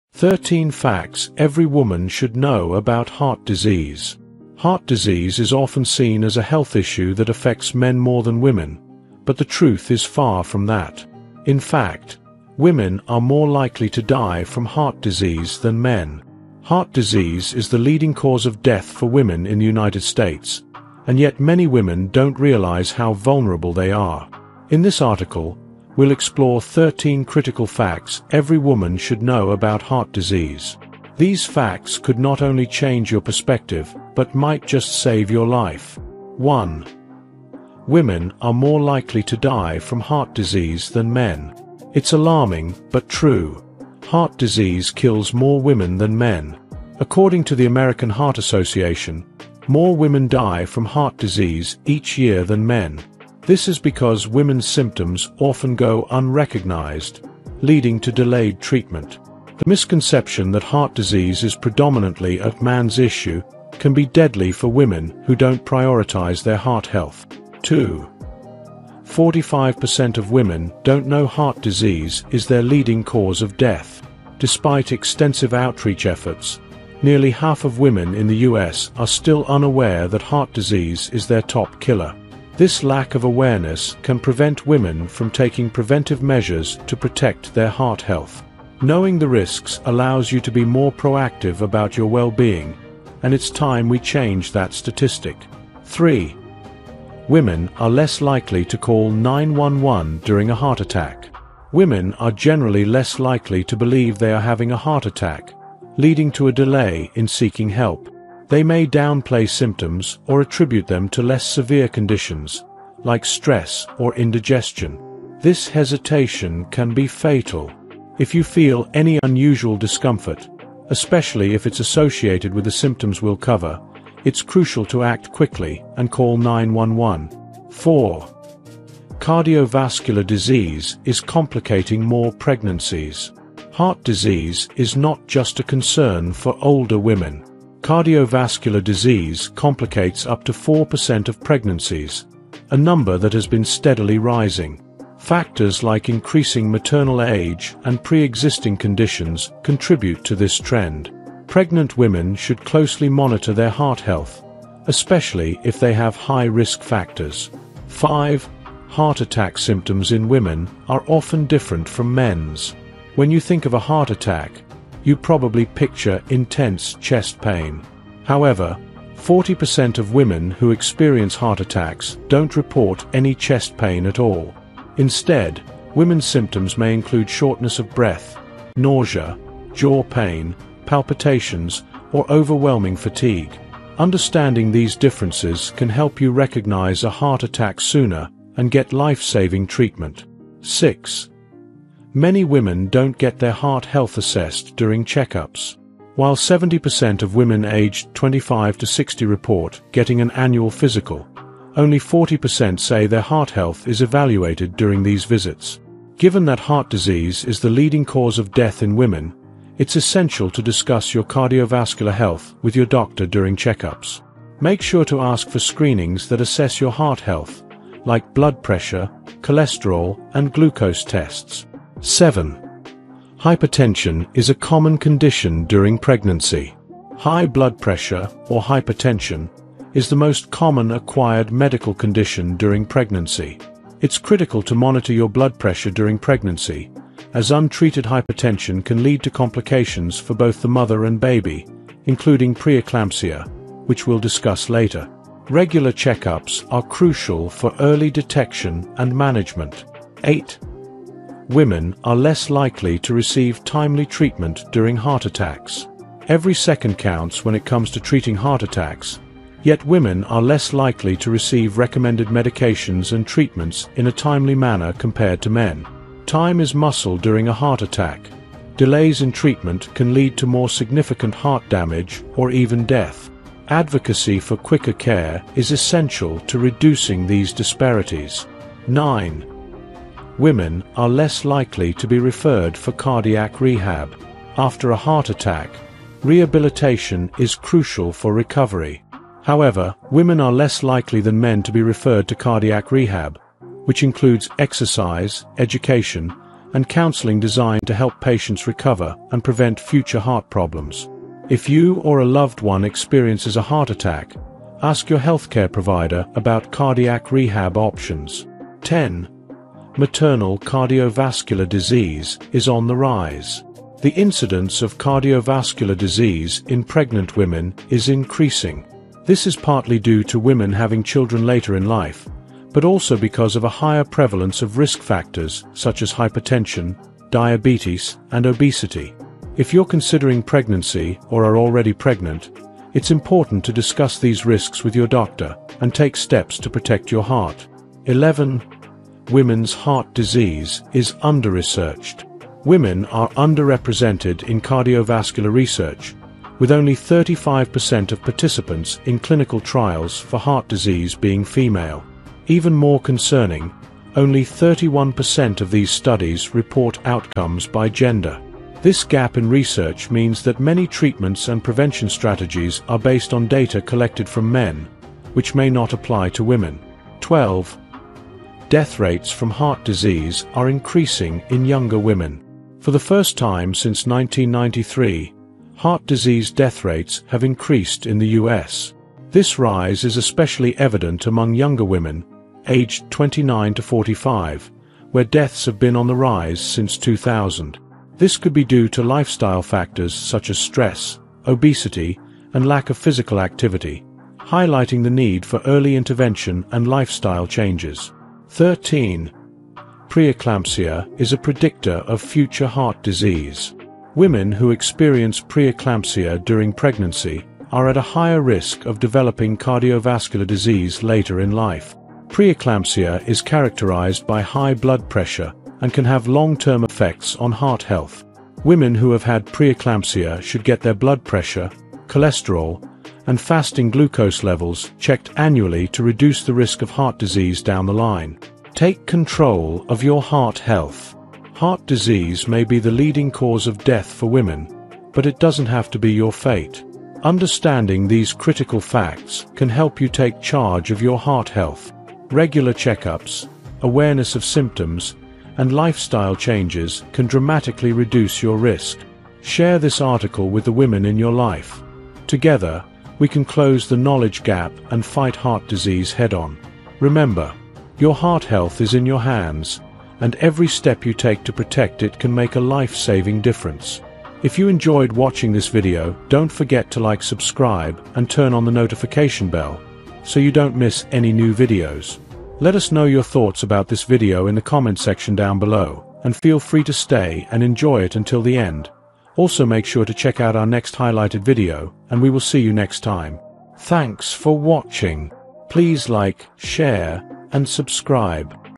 13 Facts Every Woman Should Know About Heart Disease | Know for Healthier Life | Psychological Facts
13 Facts Every Woman Should Know About Heart Disease | Know for Healthier Life | Psychological Facts
Heart disease is often perceived as a "man’s issue," but did you know that it's actually the leading cause of death for women? Many women don’t realize how vulnerable they are to cardiovascular disease, and some may even downplay symptoms that could be life-threatening. In this video, we reveal 13 crucial facts every woman needs to know about heart disease to stay informed and protect her health. Understanding these facts could save your life or the life of a loved one.

Here’s what you’ll learn in this video:
Women Are More Likely to Die from Heart Disease Than Men
Surprisingly, heart disease kills more women than men every year. Raising awareness about this critical issue is essential to reducing these alarming statistics.

Nearly Half of Women Don’t Know Heart Disease Is Their Top Killer
Despite awareness campaigns, 45% of women are still unaware that heart disease is the number one threat to their health. Learn why it’s so important to know this fact and take proactive steps.

Women Are Less Likely to Call 911 During a Heart Attack
Women often delay seeking help during a heart attack, putting their lives at greater risk. Discover how to recognize the unique heart attack symptoms in women and why every second counts.

Cardiovascular Disease Complicates More Pregnancies
Heart disease isn’t just a concern for older women. It complicates up to 4% of pregnancies, and this number is rising. If you’re pregnant or planning to be, this is vital information.

Heart Attack Symptoms in Women Differ from Men’s
Think heart attacks always come with chest pain? Think again. Many women experience different symptoms such as shortness of breath, nausea, jaw pain, and fatigue. Knowing these signs could save your life.

Few Women Get Their Heart Health Assessed Regularly
Even though 70% of women aged 25-60 get annual checkups, only 40% receive a heart health assessment. We’ll explain why it’s crucial to prioritize heart health during doctor visits.

Hypertension is a Common Condition During Pregnancy
High blood pressure is the most common acquired condition during pregnancy and should not be overlooked. Discover why managing hypertension is vital for both mother and baby.

Women Receive Delayed or Inadequate Treatment During Heart Attacks
Women are less likely to receive the recommended medications during a heart attack, leading to more severe damage. Learn why timely intervention is critical and how to advocate for your care.

Women Are Less Likely to Be Referred for Cardiac Rehab
Cardiac rehabilitation is a life-saving process, but women are often overlooked for this treatment. We’ll share why you should ask your healthcare provider about rehab options after a heart attack.

Maternal Cardiovascular Disease is on the Rise
As women have children later in life, the risk of cardiovascular disease during pregnancy increases. Learn how to protect your heart if you're planning a pregnancy.

Women Are Underrepresented in Heart Disease Research
Did you know that only 35% of participants in heart disease clinical trials are women? This gap in research affects treatment outcomes. We'll discuss the importance of advocating for more inclusive research.

Heart Disease Death Rates Are Increasing in Young Women
Heart disease deaths in women aged 29-45 are on the rise. We’ll uncover the possible reasons for this trend and share tips to reverse these statistics.

Pre-eclampsia is a Predictor of Future Heart Disease
Women who’ve experienced pre-eclampsia are at a higher risk of cardiovascular issues later in life. We'll guide you on what screenings and lifestyle changes to adopt if you’ve had pre-eclampsia.